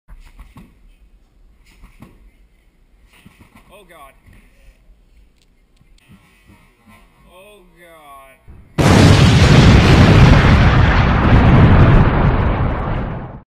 Aidan misses the foam pit
misses the foam pit and takes a tumble